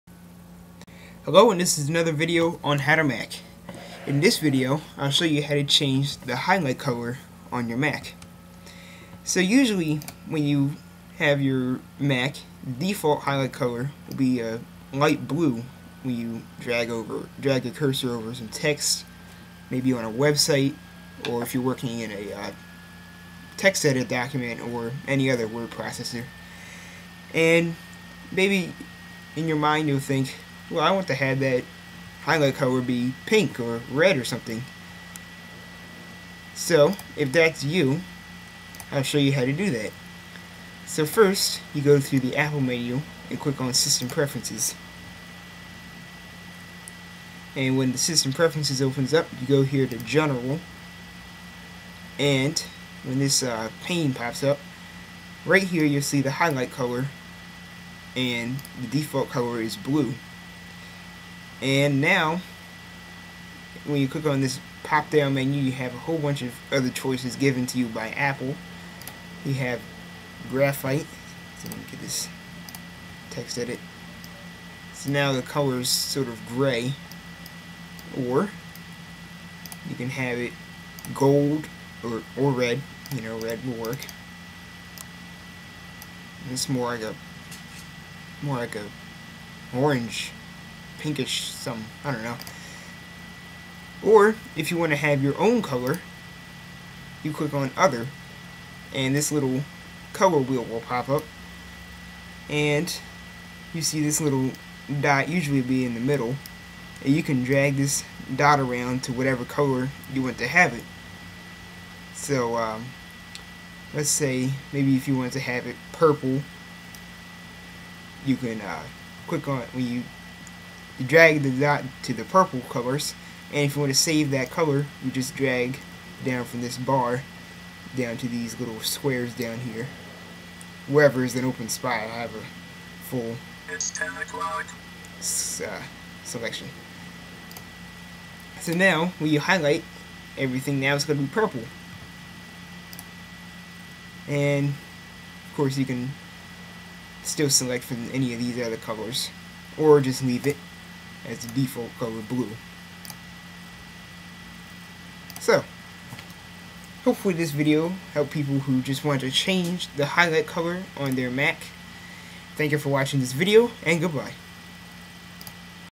How to Change the Highlight Color in OS X – Wind5387
In this video, I'll show you how to change the highlight color on your Mac.

I'm not sure why the audio is a bit off. :/